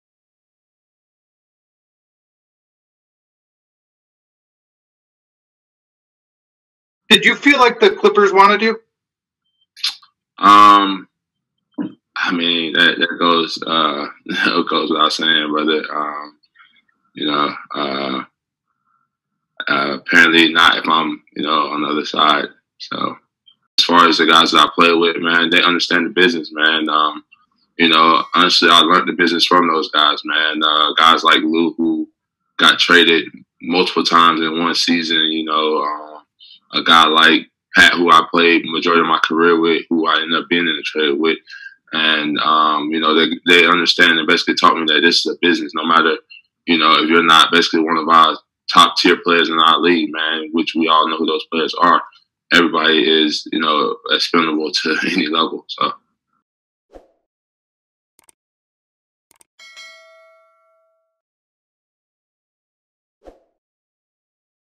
Montrezl Harrell on Clippers not wanting him, teammates reactions to joining Lakers
- Montrezl Harrell surprised the basketball world on opening night of free agency by agreeing to a deal with the Los Angeles Lakers. In leaving close friend Lou Williams and the LA Clippers for their crosstown rivals, Harrell even drew some surprising reactions from his former teammates in Patrick Beverley as well as Sindarius Thornwell.
- Among the questions asked was if Harrell felt the Clippers wanted him back to the team.